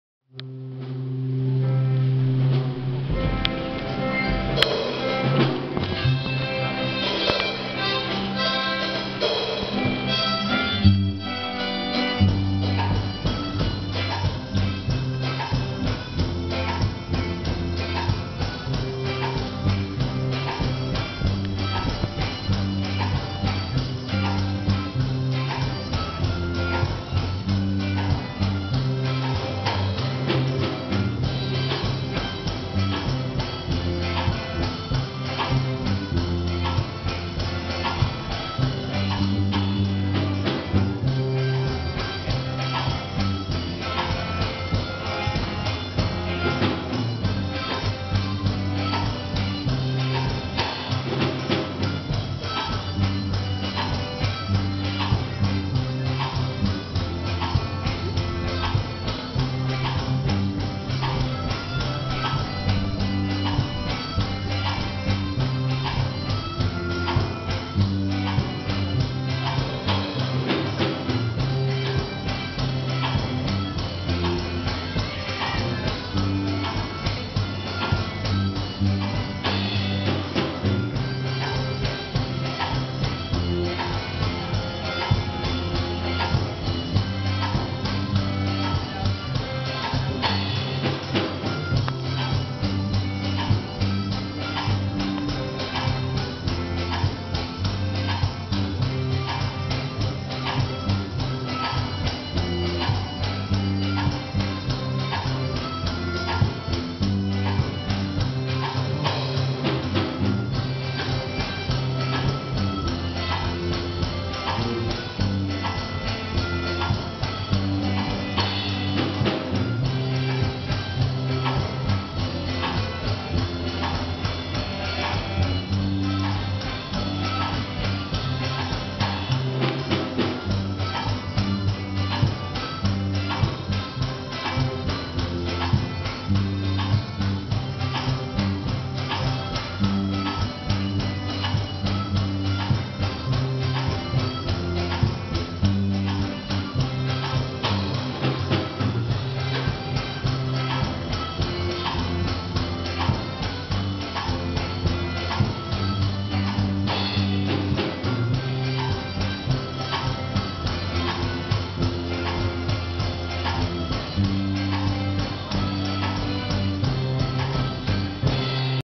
Strange Fire Introduction, Session 2 Introduction Hymn Feb 22. 2015 Rudy Plays Harmonica
Strange Fire Session 2 February 22, 2015 Introduction Harmonica.